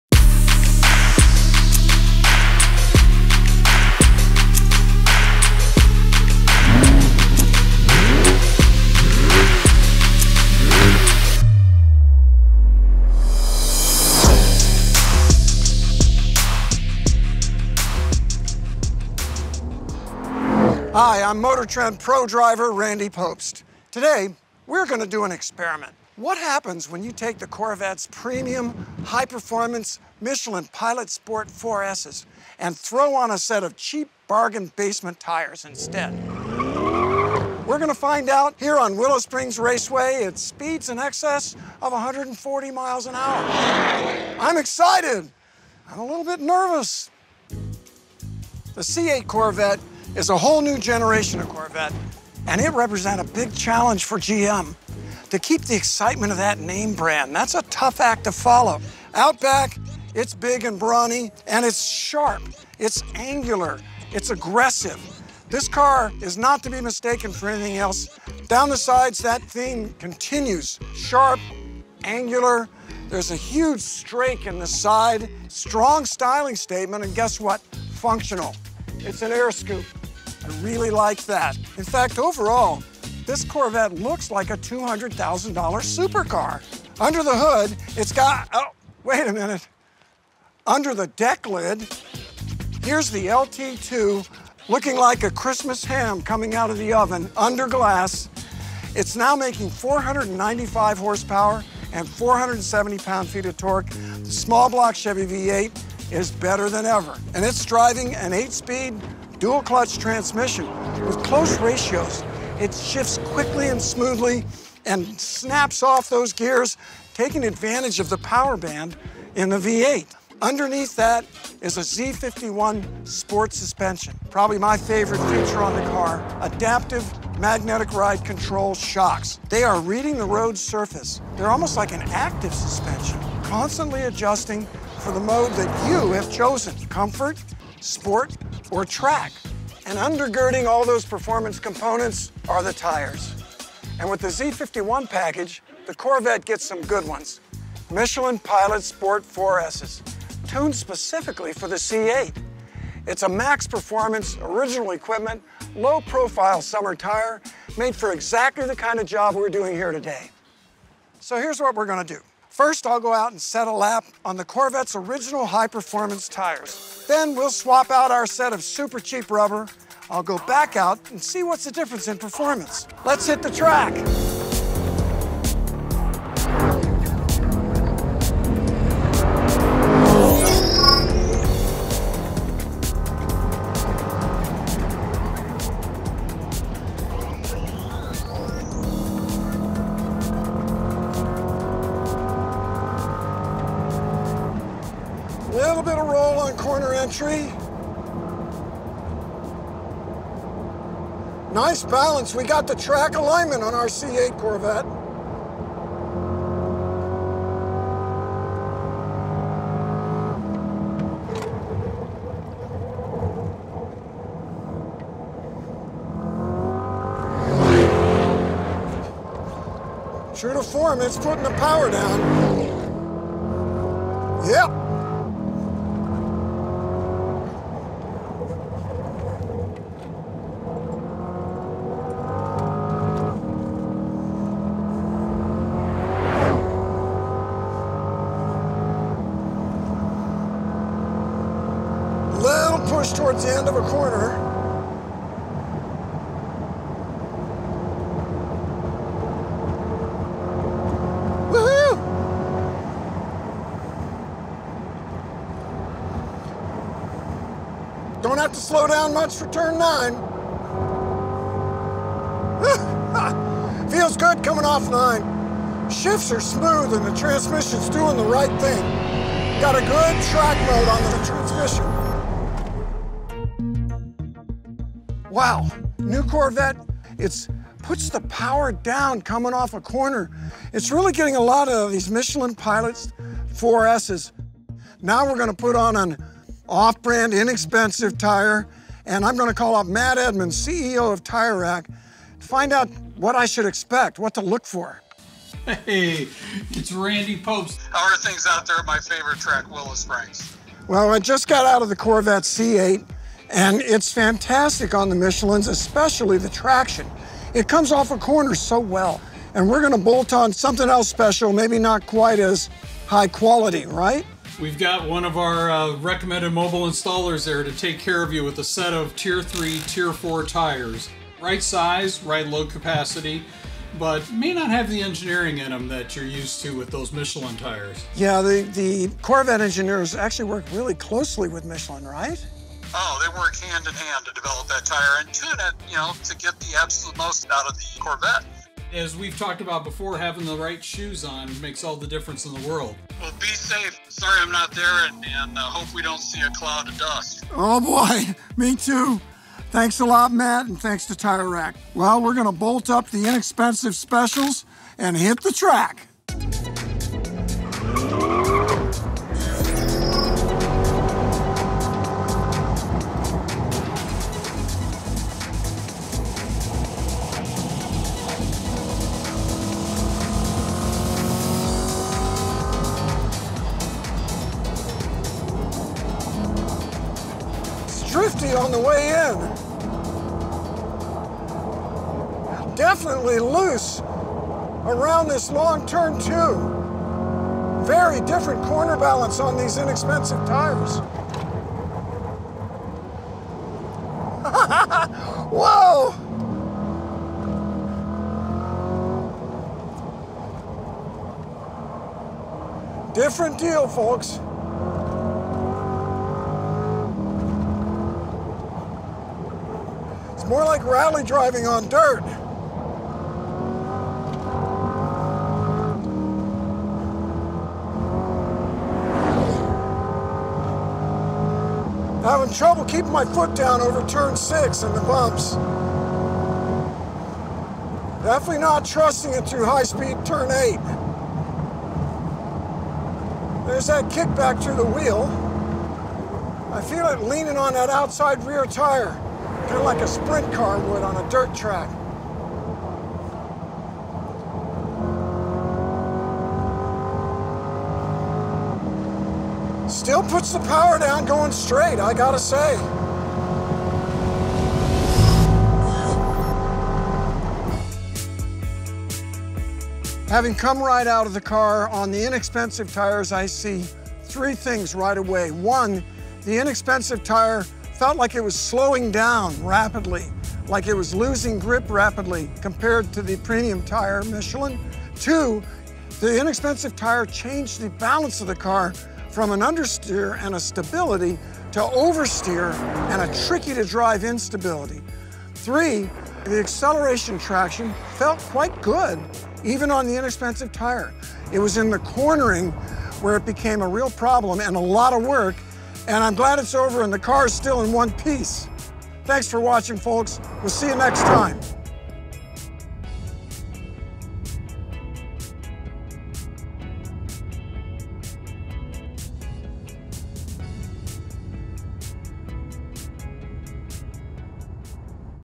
2020 Chevrolet Corvette Stingray Z51 | Tire Rack Hot Lap
Motor Trend’s Randy Pobst uses the power of the 2020 Chevrolet Corvette Stingray Z51 for an interesting experiment at Willow Springs International Raceway. How does replacing the Corvette’s premium Michelin Pilot Sport 4S tires with a generic, lower grade tire affect performance? Watch to find out.